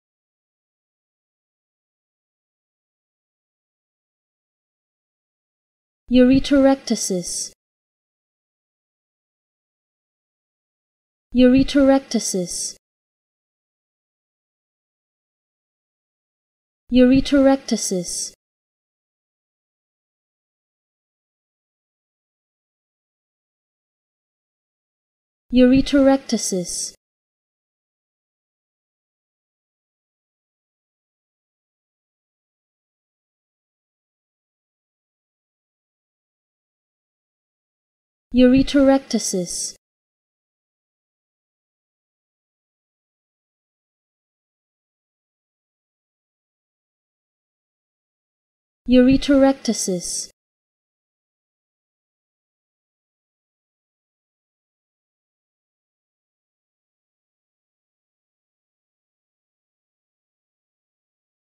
How To Say Ureterectasis
Learn how to say Ureterectasis with EmmaSaying free pronunciation tutorials.